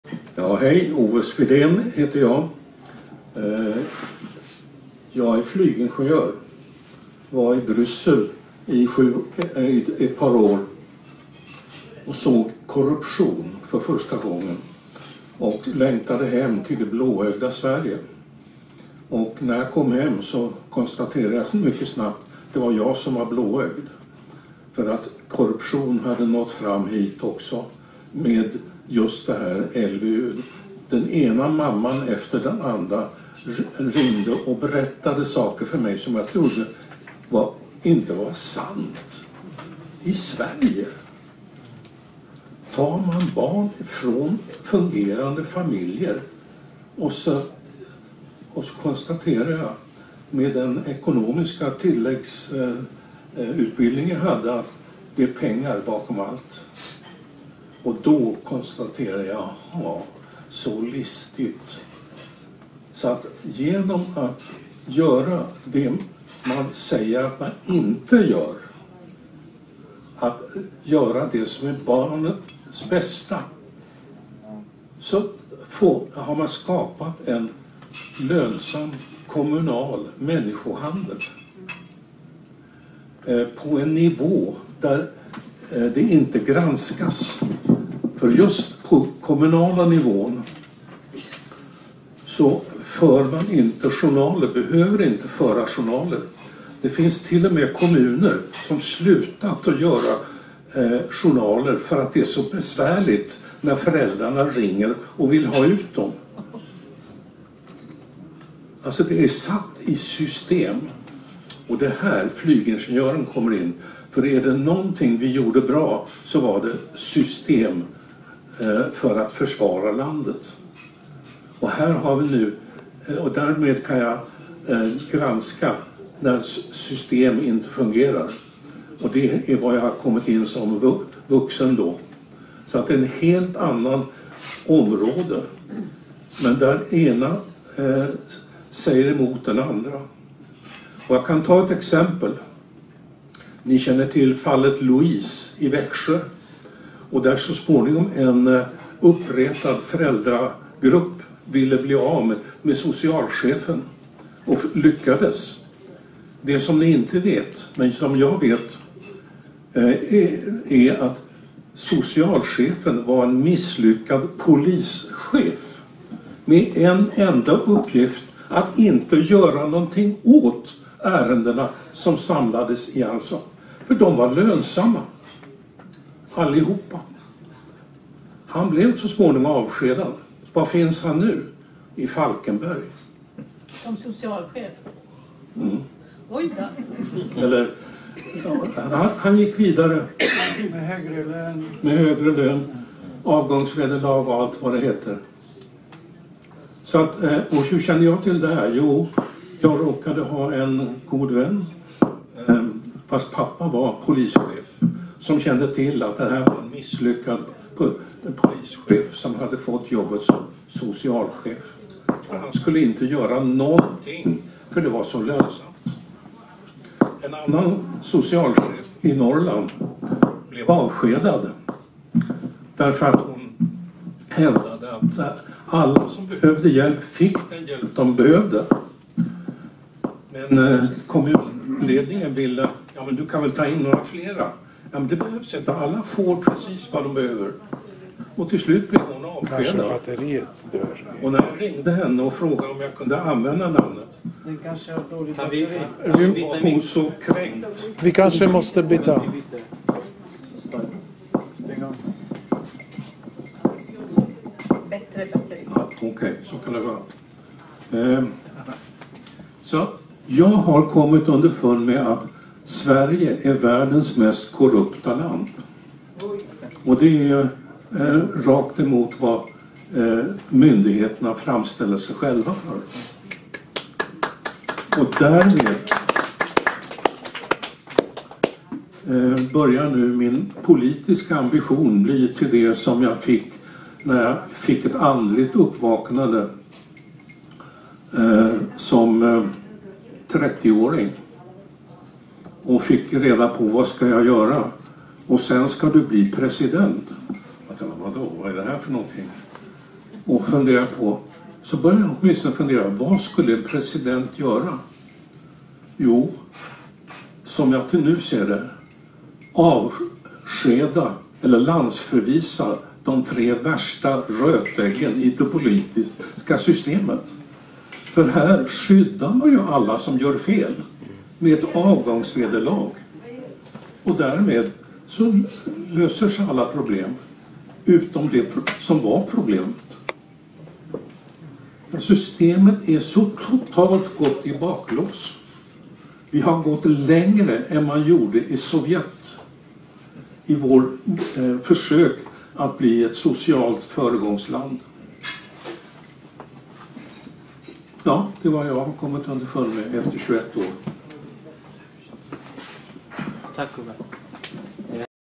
Ove Sviden, om att Sverige är världens mest korrupta land.
lönsam kommunal människohandeln, bedrivs av de svenska kommunerna genom att göra det man inte gör dvs bryr sig om barnens bästa. Utan granskning, på kommunal nivå, utan att ens föra journaler som besvärliga föräldrar kan begära utt.
Social cheff som avskedas får högre lön, avsked vederlag och bättre psition , blir befodrad uppåt för begågna brott.
Sverige mest korrupta land !
Tre gångna premierminstrar borde vara landförvissade, för förrederi.